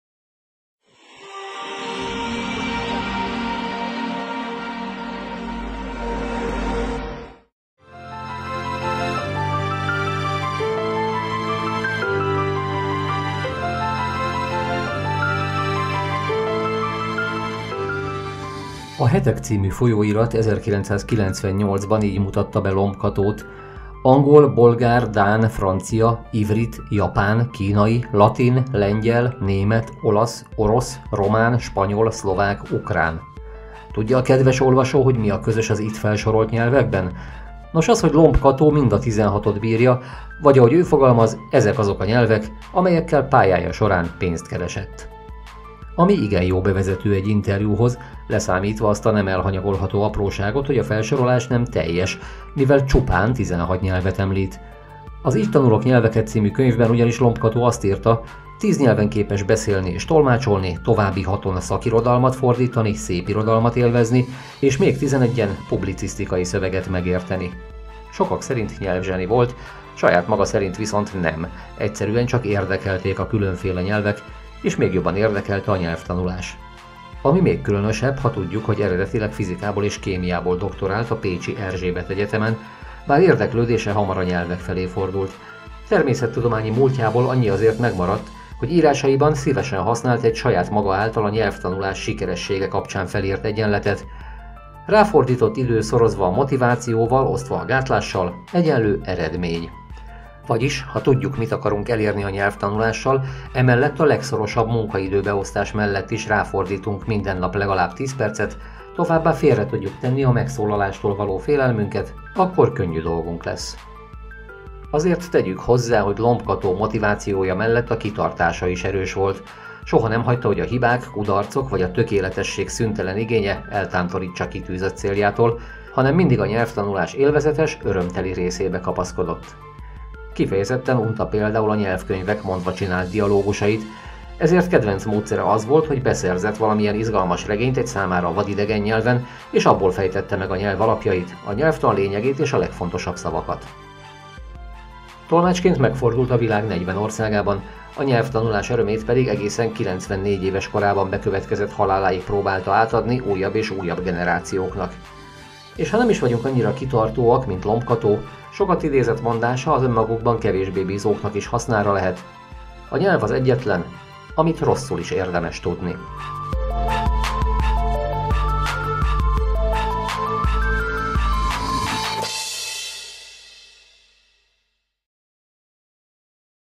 „A nyelv az egyetlen, amit rosszul is érdemes tudni!”
Van egy generáció – talán kettő is – amelynek tagjai minden alkalommal megemlítették a nevét, ha a nyelvtanulás került szóba. Ami nem is csoda, hiszen ha valaki huszonnyolc nyelvet ismer, minimum a szövegértést lehetővé tevő szinten, az bizony legalábbis említésre méltó. 2003. június 9-én hunyt el Lomb Kató.